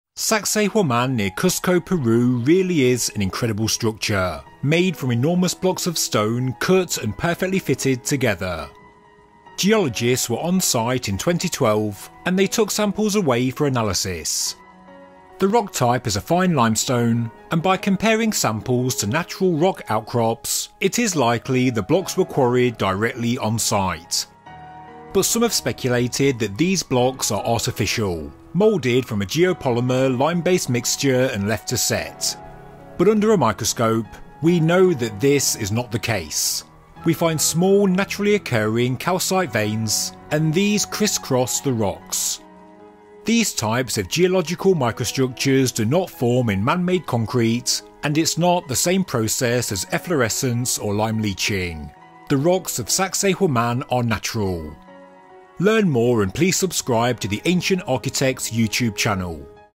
Mystery Solved! Sacsayhuaman: Geopolymer Concrete or Natural Rocks? 🧐
Sacsayhuaman, near Cusco Peru, is an incredible structure made up of huge blocks of perfectly cut and fitted stones.

Geologists were on site in 2012 to take samples and do analysis. The rock type used is a fine limestone and by comparing samples to natural rock outcrops, it is very likely the wall blocks were quarried directly on site, not brought in from miles away.

But some have speculated that these blocks are artificial, moulded from a geopolymer lime-based mixture and left to set. But under a microscope, we know this cannot be the case. We find small, naturally-occurring calcite veins criss-crossing the rocks.

These types of microstructures do not form in man-made concrete. It’s not the same process as lime-leeching or efflorescence. This clears up the mystery. These rocks are natural, quarried from local outcrops.

Learn more on the Ancient Architects YouTube channel.